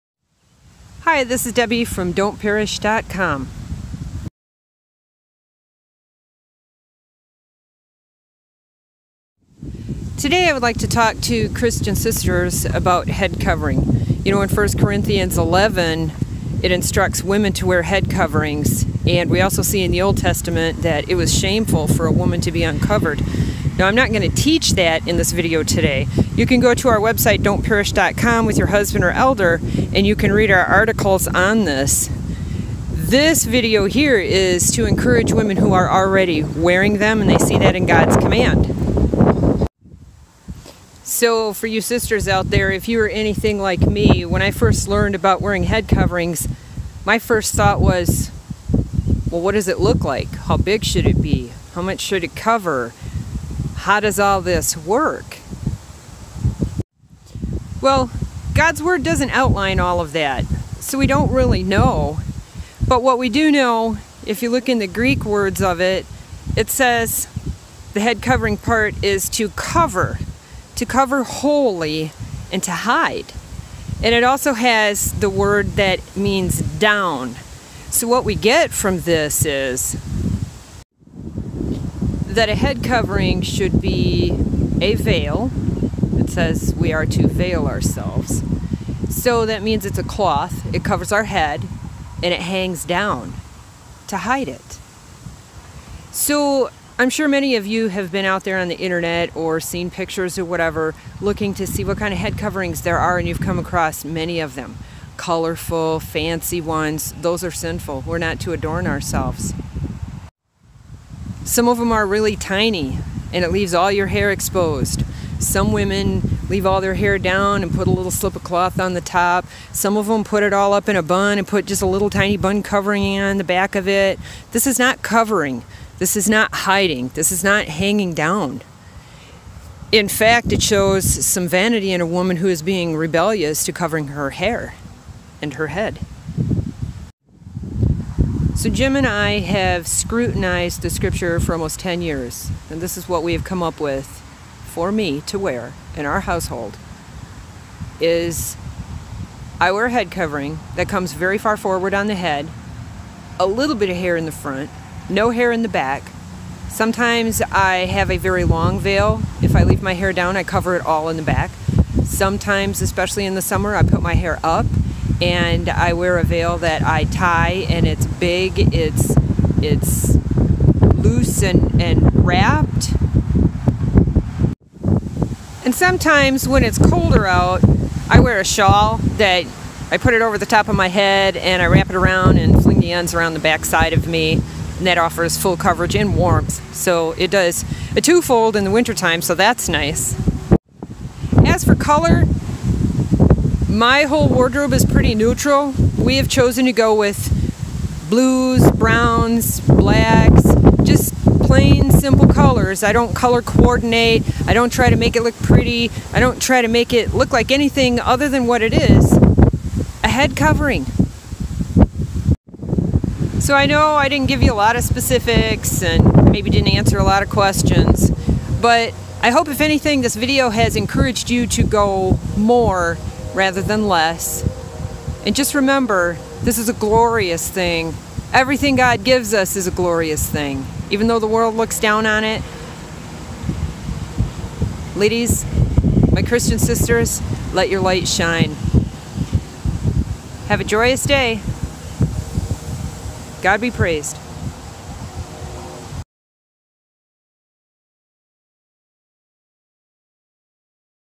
Encouragement for Christian Sisters....On Headcovering
God's Word teaches headcovering for Christian women and I simply send some encouragement on it.  Here's an article on it as well: